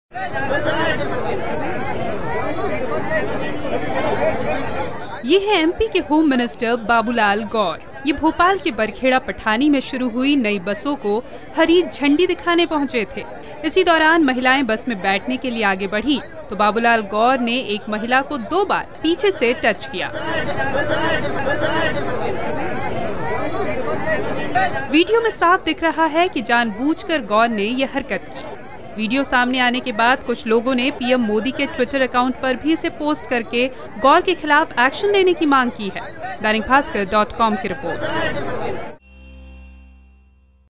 Caught On Camera: MP Home Minister Babulal Gaur Touches Woman Inappropriately
Causing embarrassment to BJP, Madhya Pradesh home minister Babulal Gaur raked up controversy again. This time the octogenarian politician was caught on camera inappropriately touching a woman at her back when she was climbing a bus. The video has evoked sharp reactions from the political and social circles with Congress and AAP burning effigy of Gaur demanding action.
Gaur, who had stirred controversies by his sexiest remarks on rape saying "rapist don't tell police before committing crime" and "rape is a social crime, sometimes it's right, sometimes it's wrong," was caught on a video on Thursday morning when he was about to flag off a "low-floor" city bus in BHEL area.